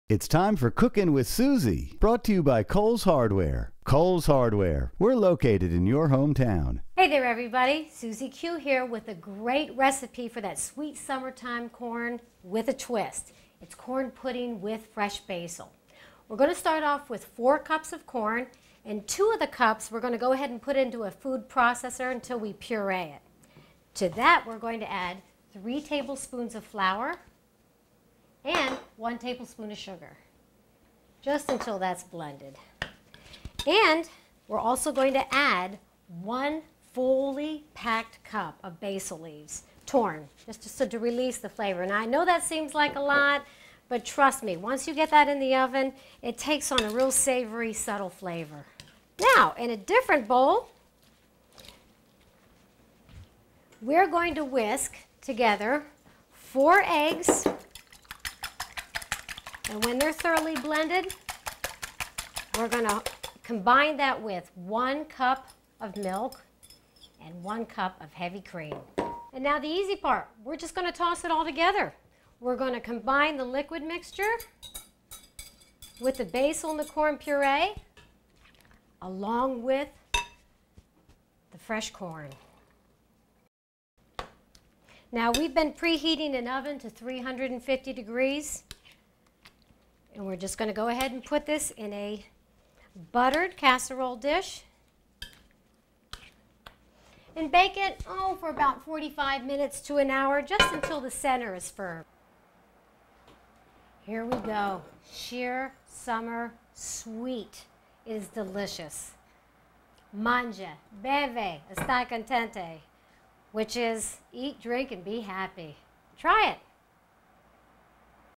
Cookin' with Suezie - Corn Pudding with Fresh Basil
Suezie Q demonstrates a great recipe for all that sweet corn using fresh basil.